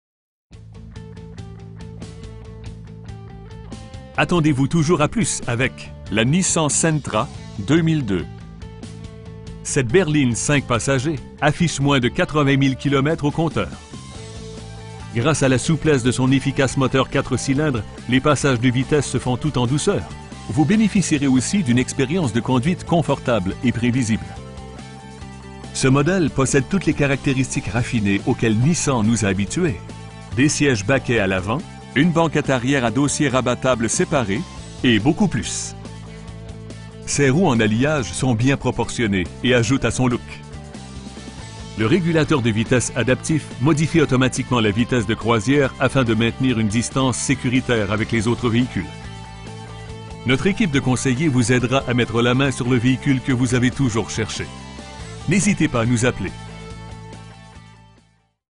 2002 Nissan Sentra GXE in Montreal, QC H2H 2L9
Nissan Prestige Iberville
4777 Rue Iberville
in Montreal, QC H2H 2L9

Attendez-vous toujours �� plus avec la Nissan Sentra 2002. Cette berline 5 passagers affiche moins de 80 000 km au compteur. Gr��ce �� la souplesse de son efficace moteur 4 cylindres, les passages de vitesses se font tout en douceur, vous b��n��ficierez aussi d'une exp��rience de conduite confortable et pr��visible. Ce mod��le poss��de toutes les caract��ristiques raffin��es auxquelles Nissan nous a habitu��s: des si��ges baquets �� l'avant, une banquette arri��re �� dossiers rabattables s��par��s, climatiseur, colonne de direction inclinable, r��troviseurs ext��rieurs �� commande ��lectrique, glaces �� commande ��lectrique, et beaucoup plus. Ses roues en alliage sont bien proportionn��es et ajoutent �� son look. Le r��gulateur de vitesse adaptif modifie automatiquement la vitesse de croisi��re afin de maintenir une distances s��curitaires avec les autres v��hicules. Notre ��quipe de conseillers vous aidera �� mettre la main sur le v��hicule que vous avez toujours cherch��. Nous serons heureux de r��pondre �� toutes vos questions. N'h��sitez pas �� nous appeler.